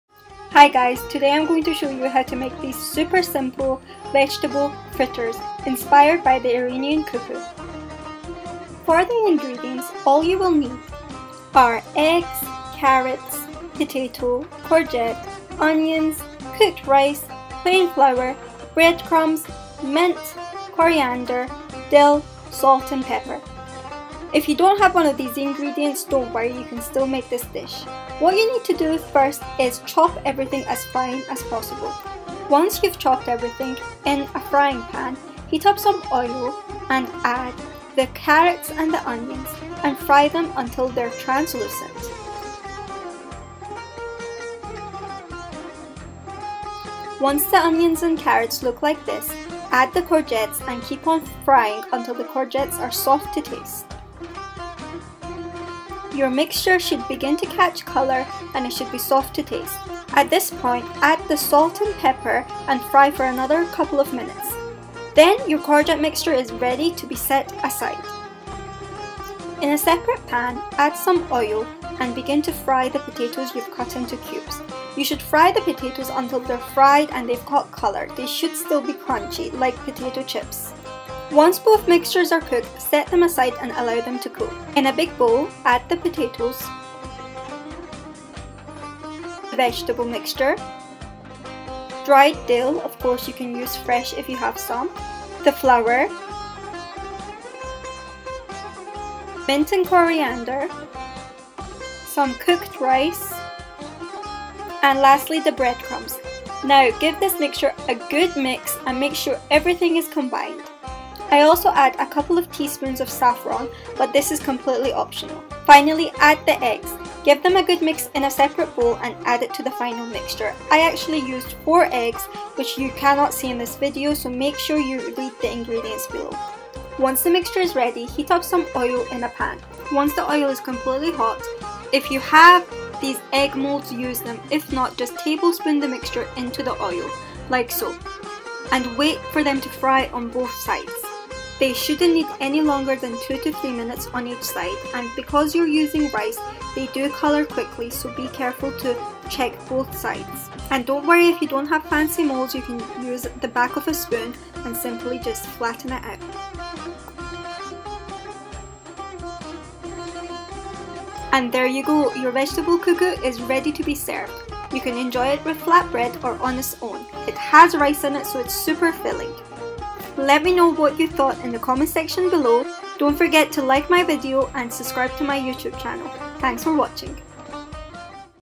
Persian Vegtable Fritters کوکو سبزیجات Kuku Sabzijat (Vegetarian)
This dish is simple, delicious and so moreish! You will keep going back for more! Its perfect for a light lunch or dinner, and it will be a hit at any dinner party!

Ingredients
1 Onion
1 Potato
1 Courgette
1 Carrot
4 Eggs
2tbsp chopped mint
2tbsp chopped coriander
2tbsp chopped dill
4tbsp white Flour
2tbsp breadcrumbs
Salt and Pepper
Oil
Saffron (optional)